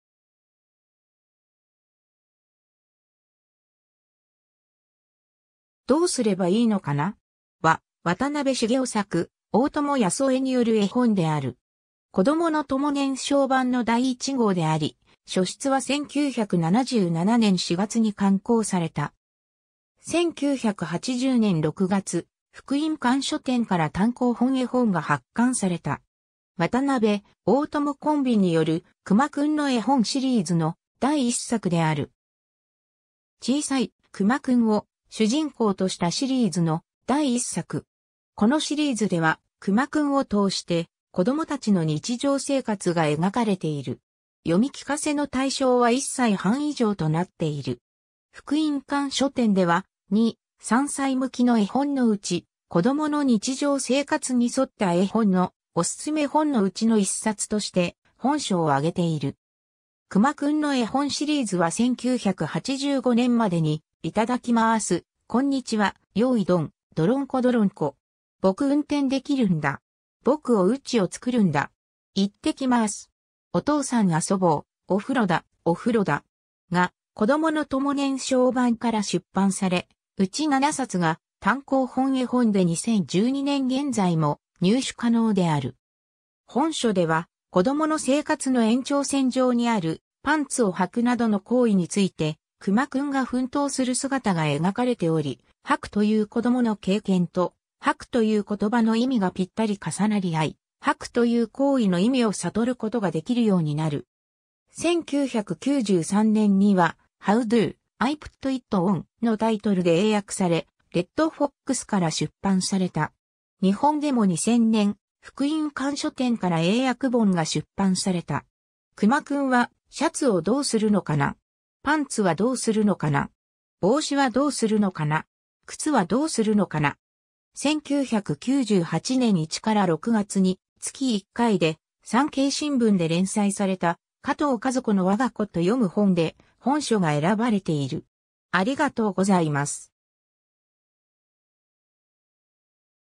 どうすればいいのかな?
『どうすればいいのかな?』は、渡辺茂男作・大友康夫絵による絵本である。
こどものとも年少版の第1号であり、初出は1977年4月に刊行された。
1980年6月、福音館書店から単行本絵本が発刊された。
渡部・大友コンビによる「くまくんの絵本」シリーズの第1作である。
 小さい「くまくん」を主人公としたシリーズの第1作。
このシリーズでは「くまくん」を通して、子どもたちの日常生活が描かれている。
読み聞かせの対象は1歳半以上となっている。
福音館書店では、2,3才向きのえほんのうち、「子どもの日常生活にそった絵本」のおすすめ本のうちの1冊として、本書を挙げている。
 「くまくんの絵本」シリーズは1985年までに『いただきまあす』『こんにちは』『ようい どん』『どろんこ どろんこ!』『ぼく うんてん できるんだ!』『ぼくおうちをつくるんだ!』『いってきまあす!』『おとうさん あそぼう』『おふろだ、おふろだ!』がこどものとも年少版から出版され、うち7冊が単行本絵本で2012年現在も入手可能である。
 本書では、子供の生活の延長線上にある「パンツをはく」などの行為についてくまくんが奮闘する姿が描かれており、「はく」という子供の経験と「はく」という言葉の意味がぴったり重なり合い、「はく」という行為の意味を悟ることができるようになる。
 1993年には、『How do I put it on?』のタイトルで英訳され、Red Foxから出版された。
日本でも2000年、福音館書店から英訳本が出版された。
 くまくんは、シャツをどうするのかな。
パンツはどうするのかな。
ぼうしはどうするのかな。
くつはどうするのかな。
 1998年1-6月に月1回で産経新聞で連載された「かとうかずこのわが子と読む本」で、本書が選ばれている。